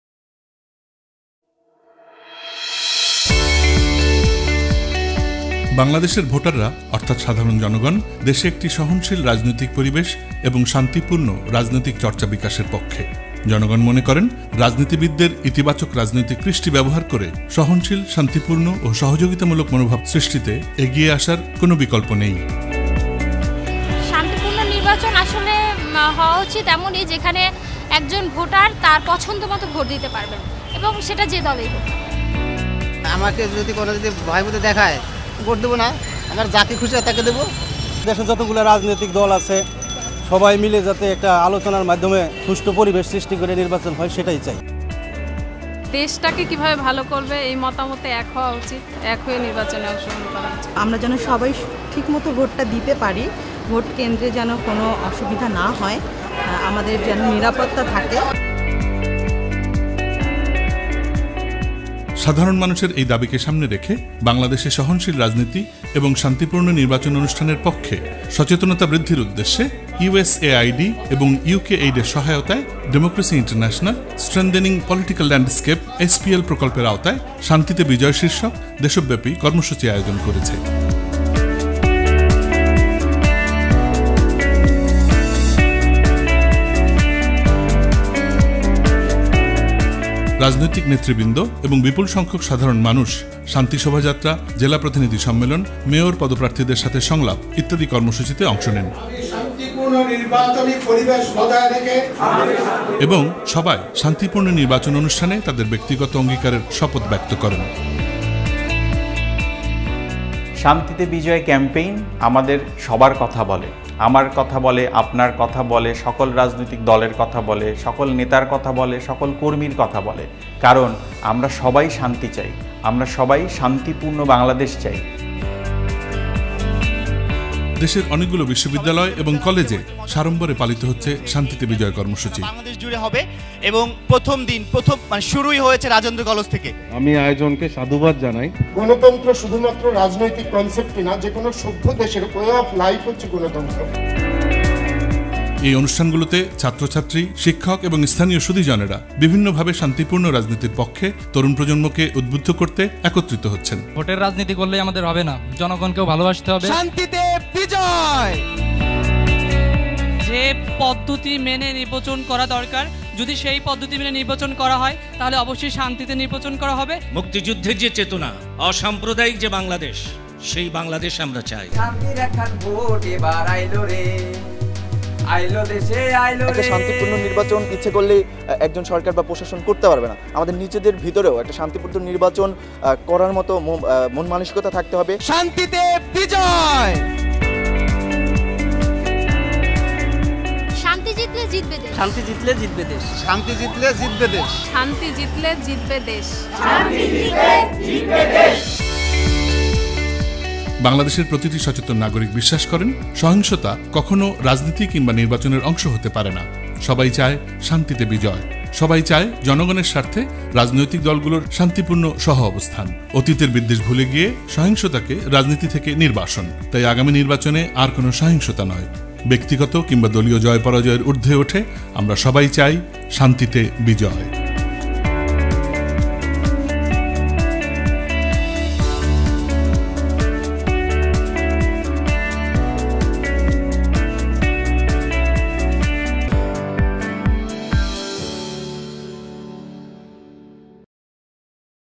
Shantite Bijoy (Win with Peace) Campaign 2018
Shantite Bijoy (Win with Peace), was a nationwide campaign to create political awareness and promote fair elections among specially the youth of Bangladesh, ahead of the 2018 national elections of the country. The campaign was organised by Democracy International, funded by USAID & UKAID.
All video productions done by Greenbee Communications